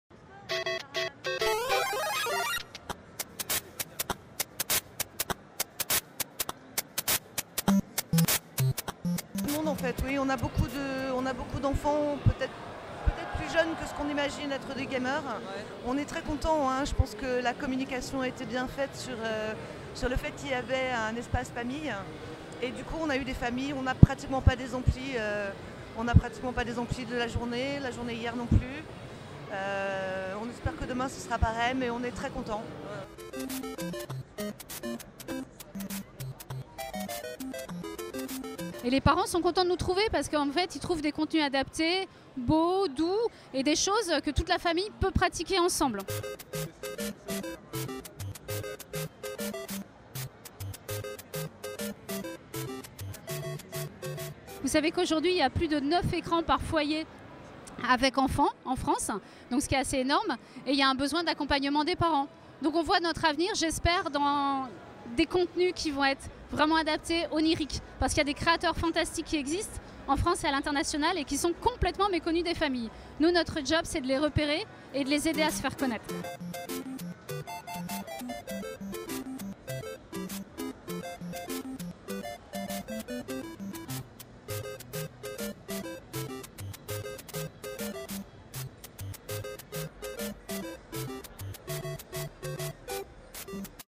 Gamers Assembly 2016 - Les enfants à la GA (Espace Famille)
Vidéo présentant l'espace famille de la Gamers Assembly 2016 GA2016.

Merci à tous de nous avoir suivis pendant la Gamers Assembly 2016.